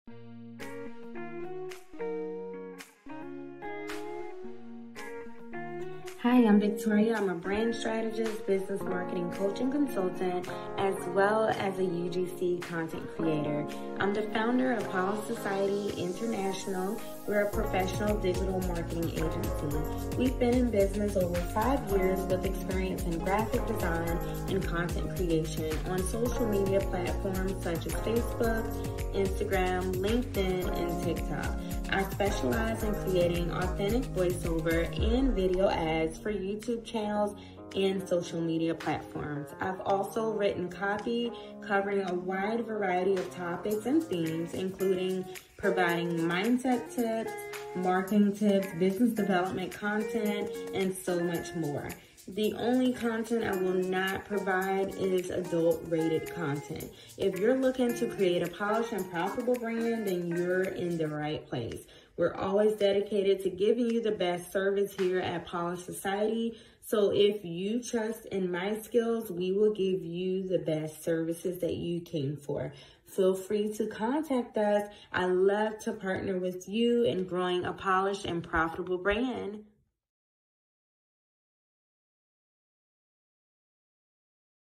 It's my goal to help you build a polished & profitable brand
Hello, Society Polishers! I'm Victoria your New Business Bestie. If you are an online entrepreneur or small business owner this is definitely the space for you!

We all have expertise & experience in something but don't know the first thing about building a business. This channel exists to help bridge the gap between transitioning from your career into entrepreneurship & business.

Each week I share videos on topics like: how to become a life coach, how to build a polished & profitable business, how to make money as a life coach, and so much more. I also share days in the life of a life coach, where I go behind-the-scenes vlog-style, showing the day-to-day duties of building a coaching business.

If you are a startup entrepreneur, and you are looking to build the confident business mindset to grow your own successful business, we can partner up to help you meet your goals -

- xo OfficialPolished